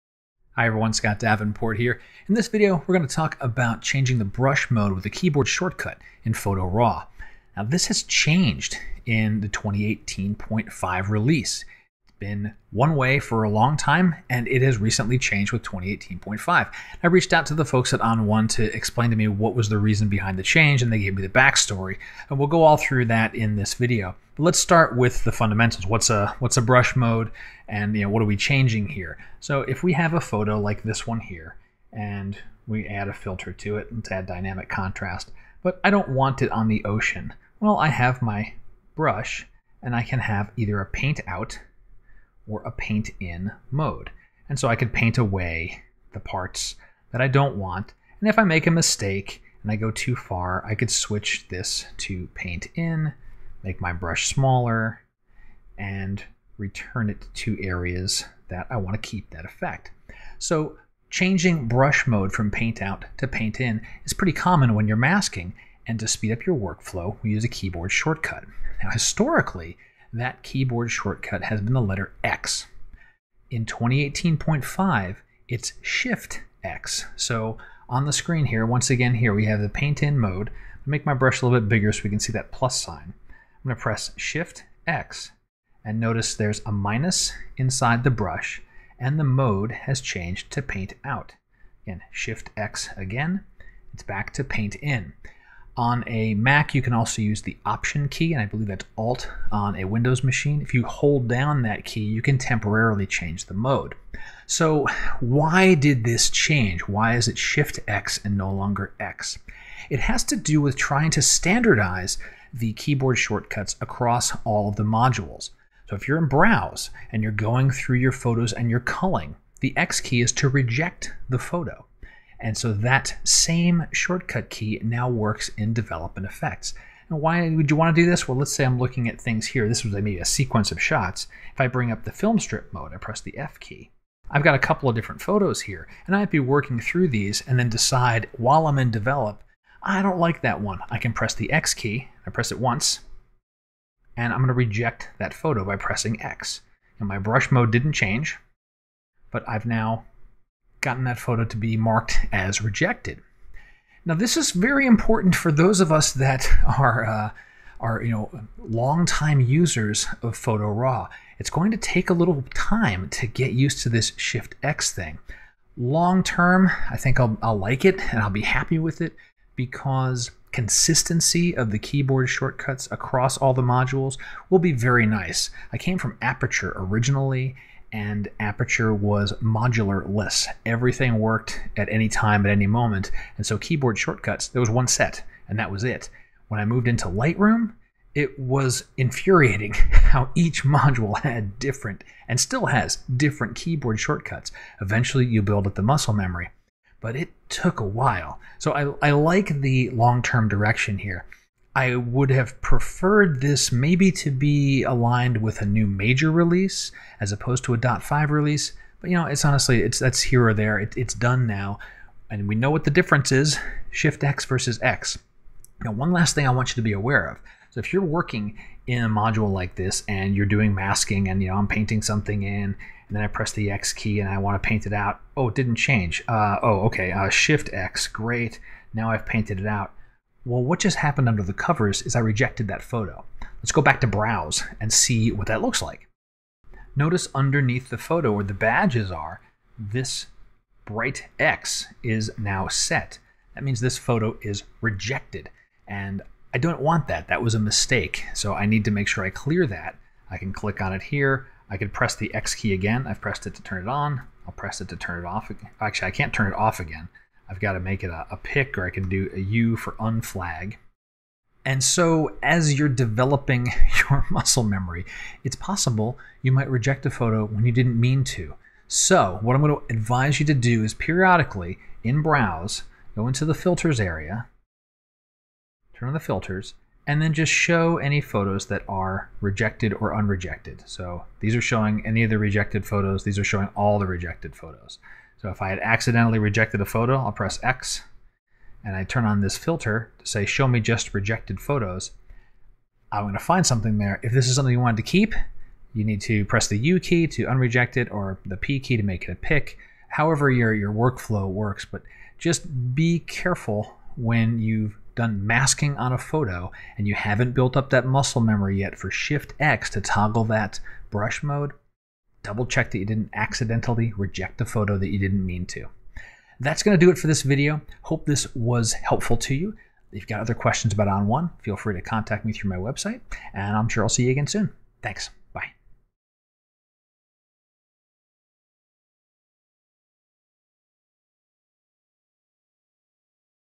ON1 Tutorial - How To Toggle The Masking Brush Mode
The keyboard shortcut to toggle the masking brush mode has changed in Photo RAW 2018.5!

For many years, the ‘x’ key toggled the ON1 brush between Paint Out and Paint In mode. This changed in Photo RAW 2018.5 to Shift-x. And there’s a reason for the change - consistency across modules. Watch this video for the backstory, and something to watch out for while you’re retraining your muscle memory for Shift-x.

SAVE 20% on your ON1 purchase.